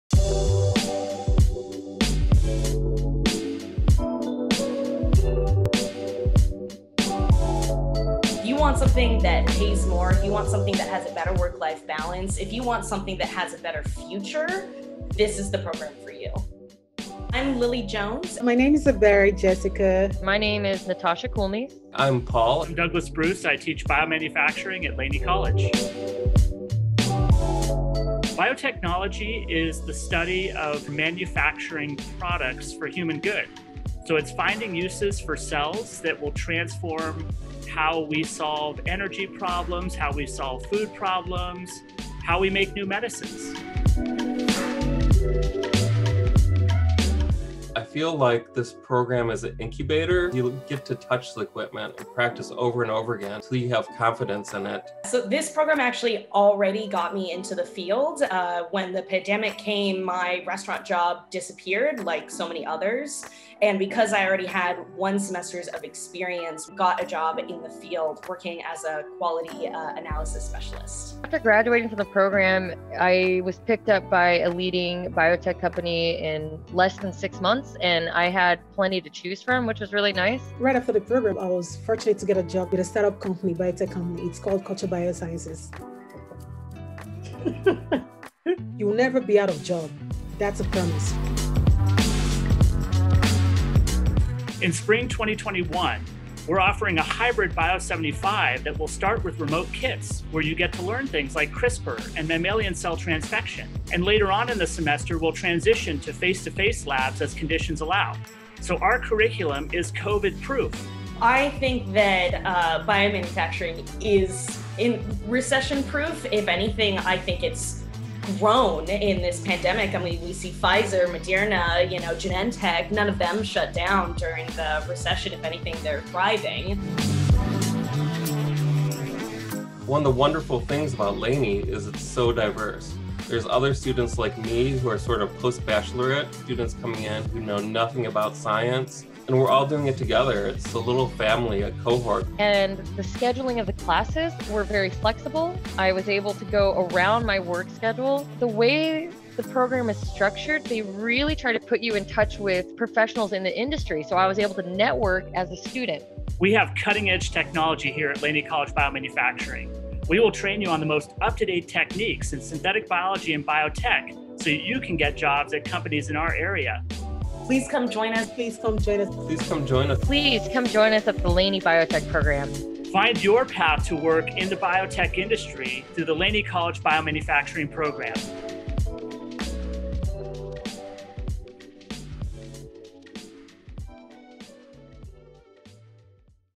Student SUCCESS STORIES from Laney Biotech Program
Heart felt Student Testimonials from our beloved alumni. Listen in as they share their experiences from our Biotech Program, and describe their new, flourishing careers in the Bay Area.

Spring 2021 Classes start SOON !